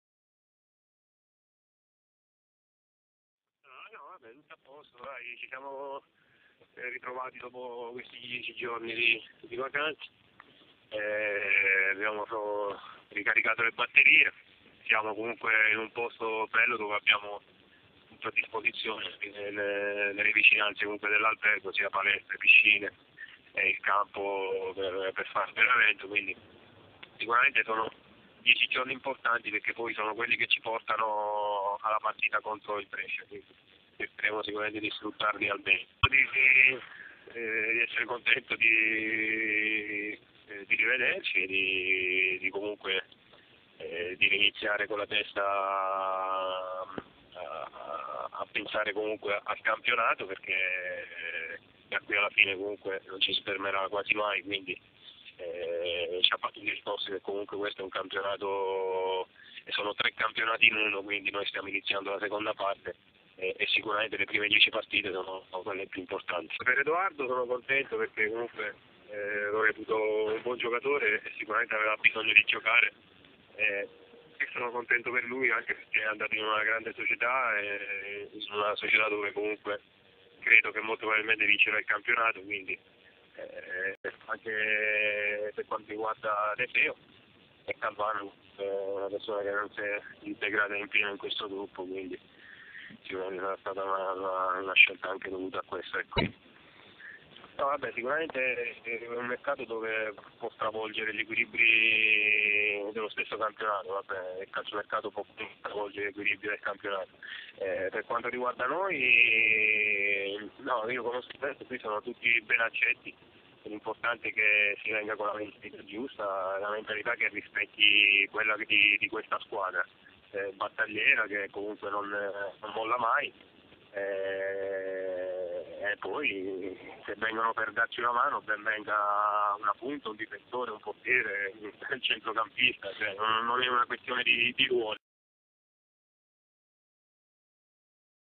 Virtus Lanciano Channel: le dichiarazioni di capitan Carlo Mammarella dal ritiro di Corato
09/01/2014 - Il capitano parla all'ufficio stampa della Virtus